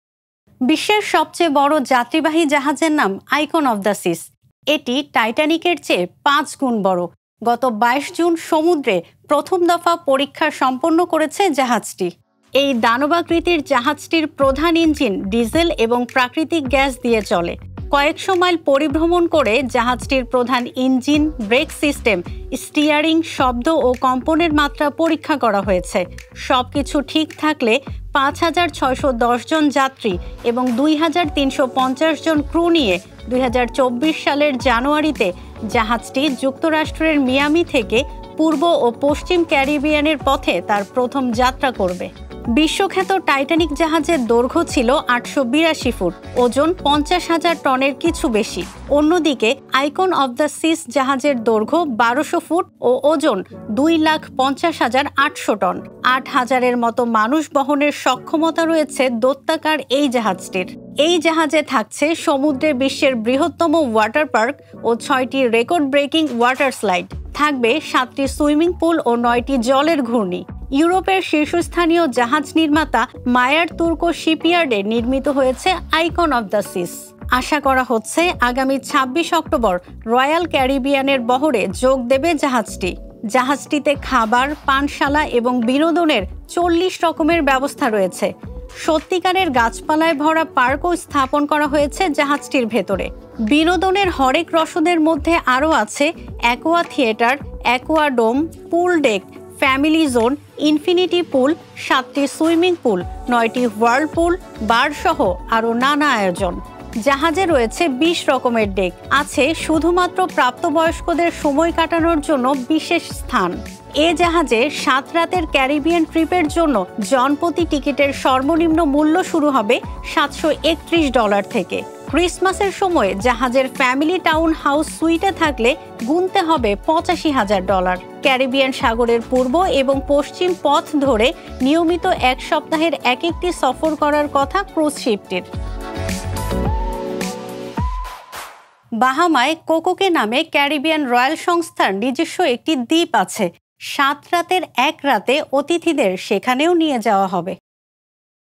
বিস্ময়কর জাহাজ ‘আইকন অব দ্য সিস’!
বিশ্বের সবচেয়ে বড় যাত্রীবাহী জাহাজের নাম ‘আইকন অব দ্য সিস’। এটি টাইটানিকের চেয়ে পাঁচ গুণ বড়। গত ২২ জুন সমুদ্রে প্রথম দফা পরীক্ষা সম্পন্ন করেছে জাহাজটি।
কয়েকশ’ মাইল পরিভ্রমণ করে জাহাজটির প্রধান ইঞ্জিন, ব্রেক সিস্টেম, স্টিয়ারিং, শব্দ ও কম্পনের মাত্রা পরীক্ষা করা হয়েছে। সবকিছু ঠিক থাকলে ৫ হাজার ৬১০ জন যাত্রী এবং ২ হাজার ৩৫০ জন ক্রু নিয়ে ২০২৪ সালের জানুয়ারিতে জাহাজটি যুক্তরাষ্ট্রের মিয়ামি থেকে পূর্ব ও পশ্চিম ক্যারিবিয়ানের পথে তার প্রথম যাত্রা করবে।